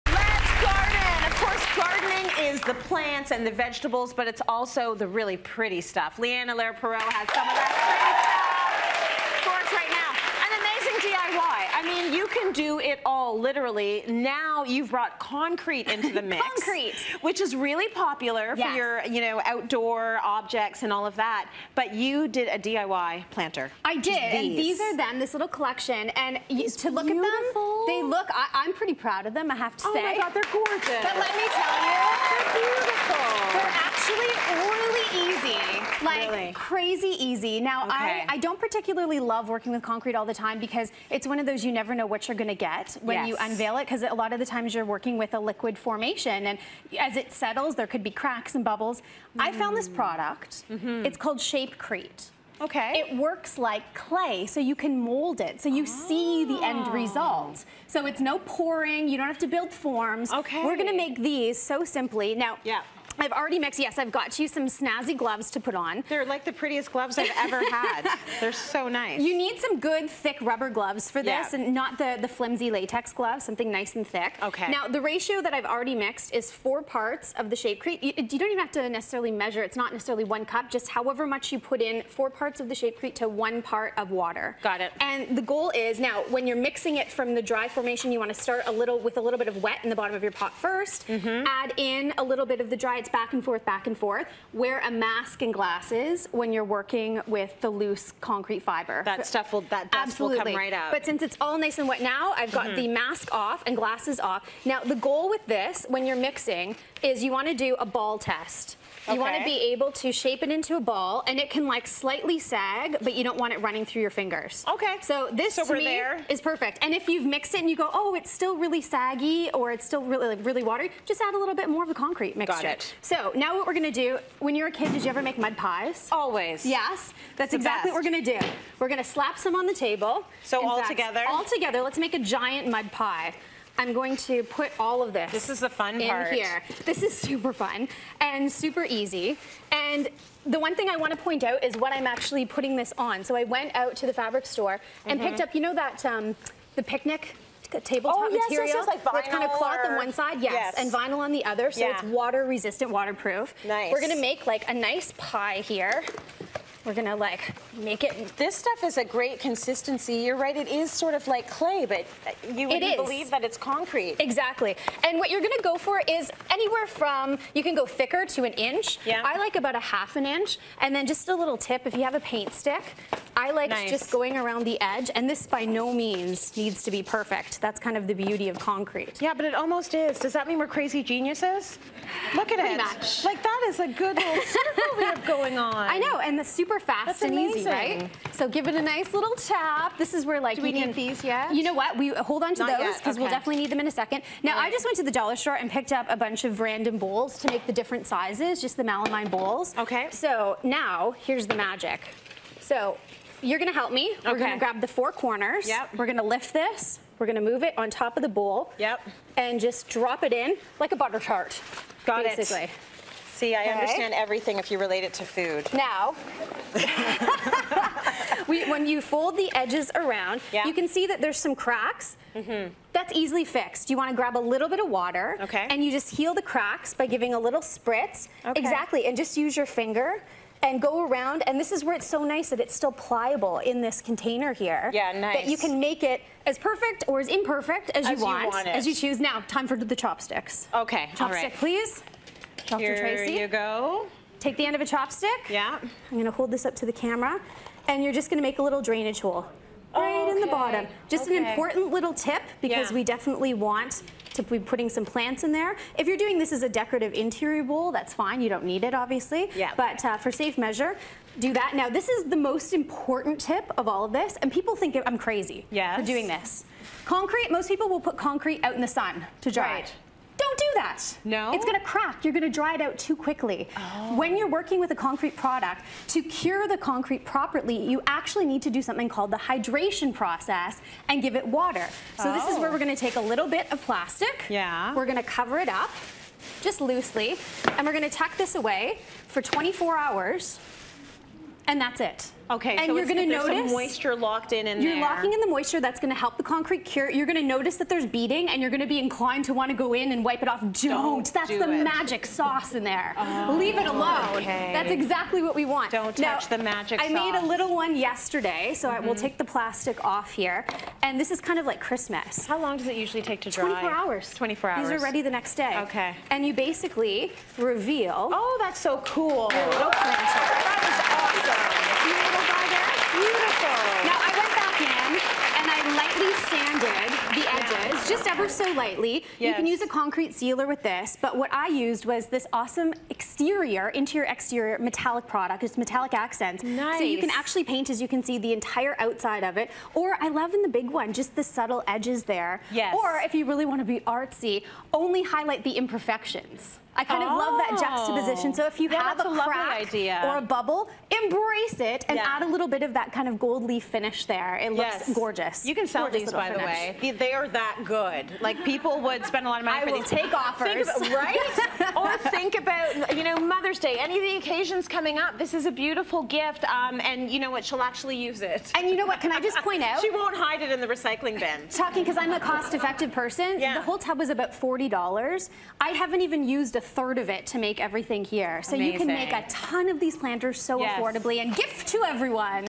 How to DIY concrete planters using only 2 materials
DIY expert, Leigh-Ann Allaire Perrault shows us how she uses concrete and paint to make
beautiful planters that can pass off as expensive store-bought products. It's makes a great idea for the perfect Mother's Day gift!